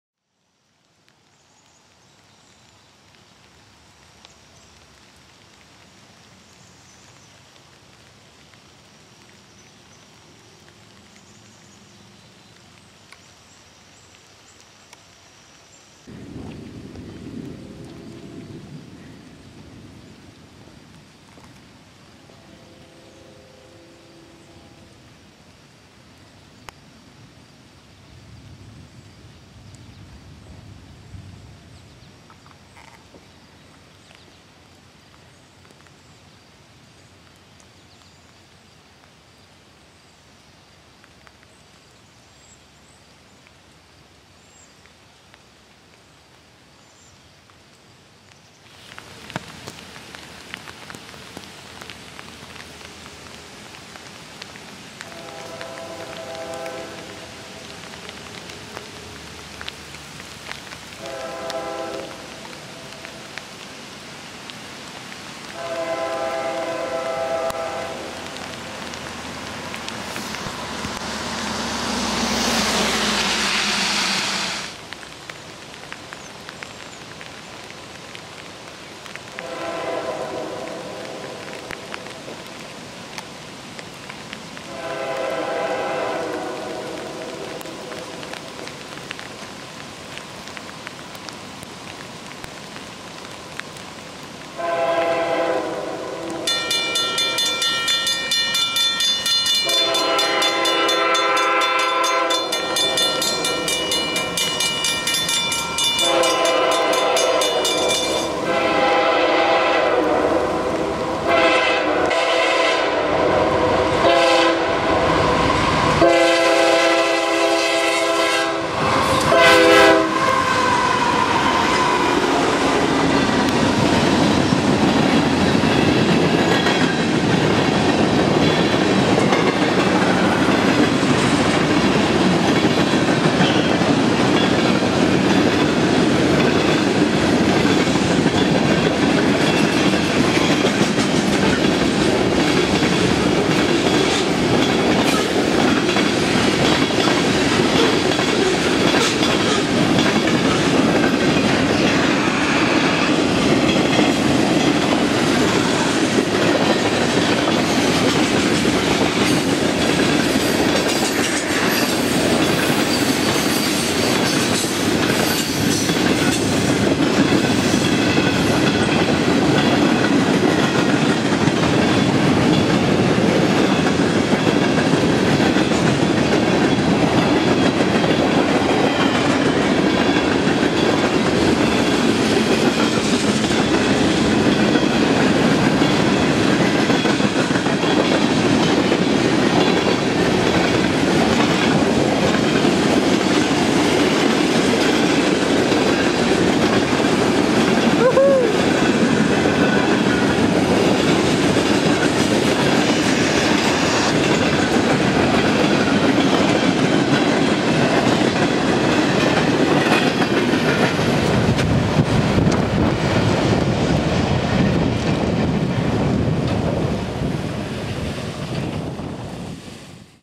CSX AC440CW 561 Leads Mixed Freight | County Road 5, Canaan, NY
CSX AC440CW 561 leads three other company units and 91 mixed cars westbound for Selkirk through the County Road 5 crossing in Canaan, NY, just a few miles from the Massachusetts border. My 100th video on YouTube,